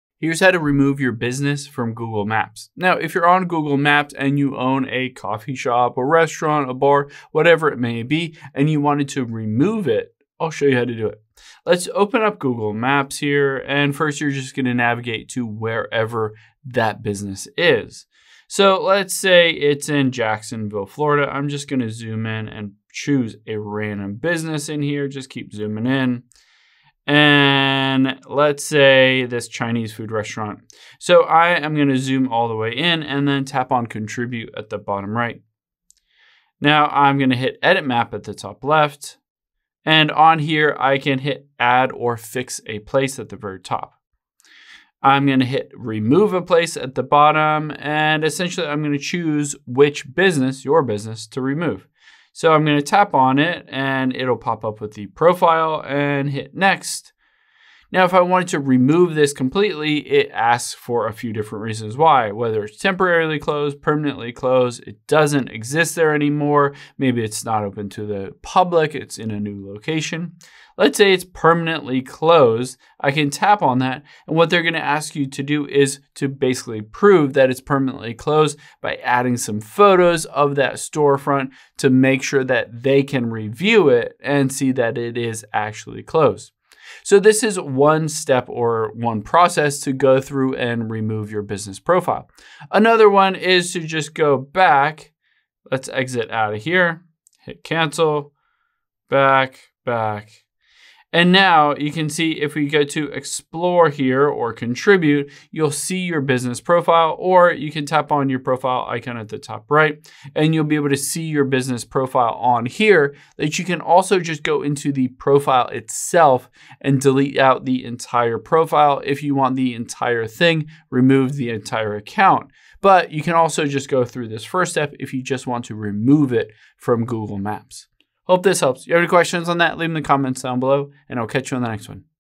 How To Remove Your Business From Google Maps
Here's how you can remove your business from Google Maps if you closed your business, want to remove the location or the whole account.